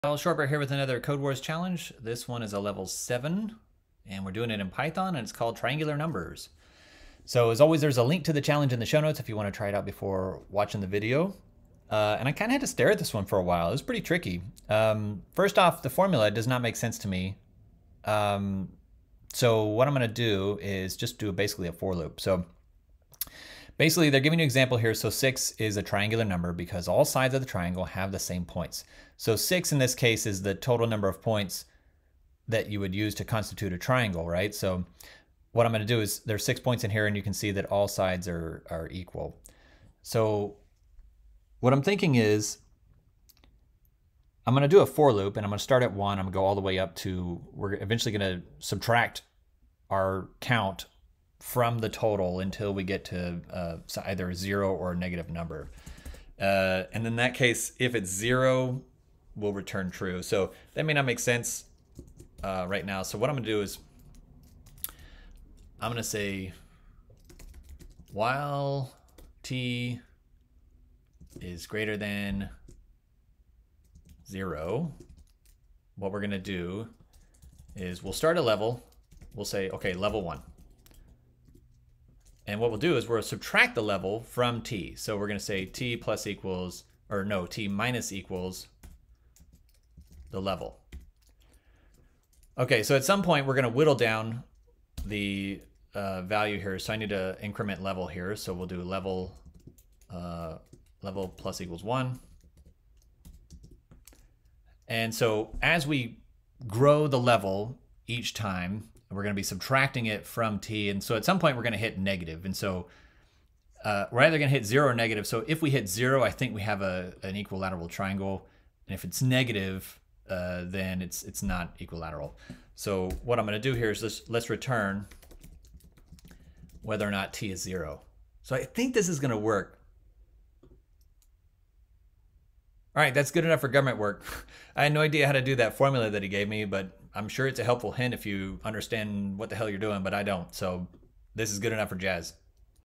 Codewars - Python - Triangular Numbers
Code along with me as we solve 'Triangular Numbers', a Level 7 kyu python codewars challenge. Here's a link to the challenge